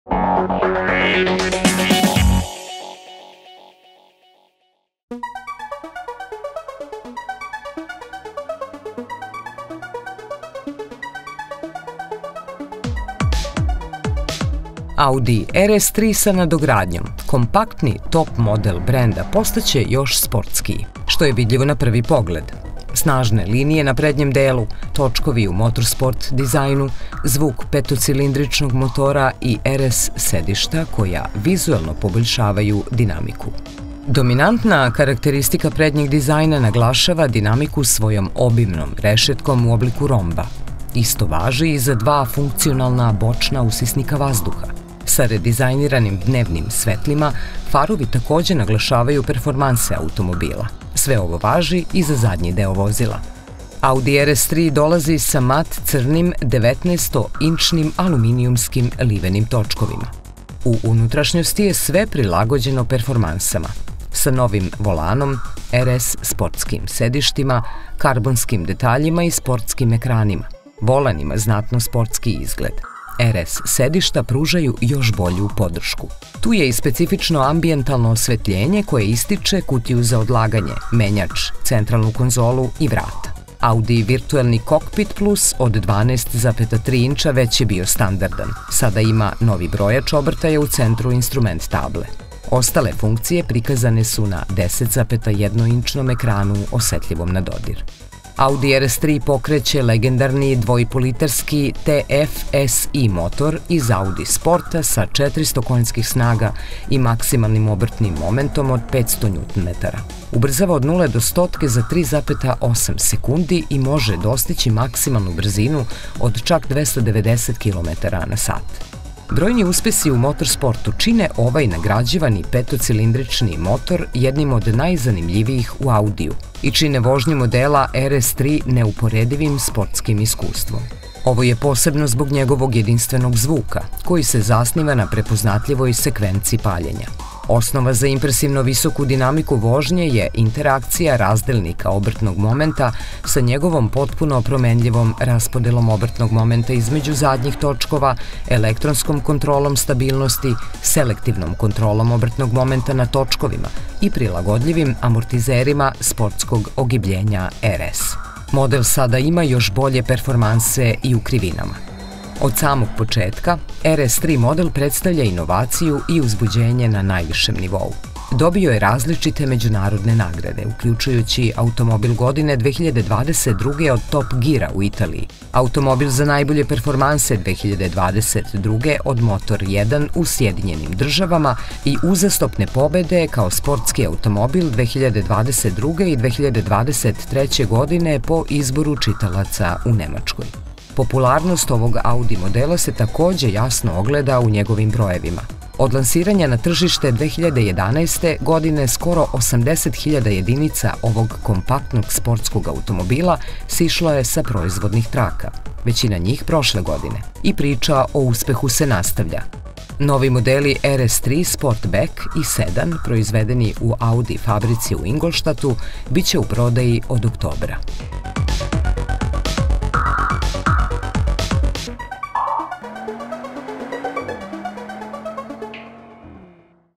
MOBIL AUTO TV - AUDI RS 3 NADOGRADNJA
Audi RS 3 sa nadogradnjom, kompaktni top model brenda, postaće još sportskijizahvaljujući ssvjim snažnim linijama na prednjem delu, točkovima u motorsport dizajnu, zvuku petocilindričnog motora i RS sedištima